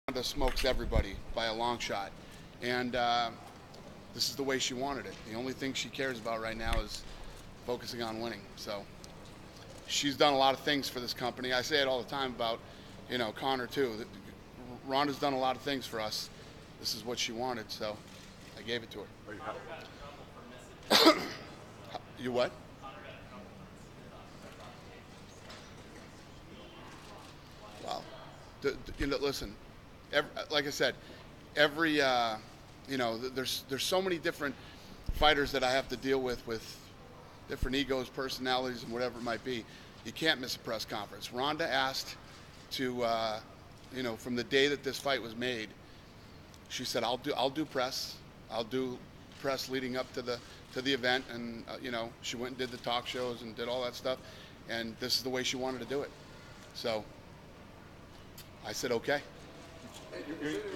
Dana White "On Ronda Not Doing Interviews"
Dana White speaks about UFC 207 Headliner Ronda Rousey not taking part in fight week interviews.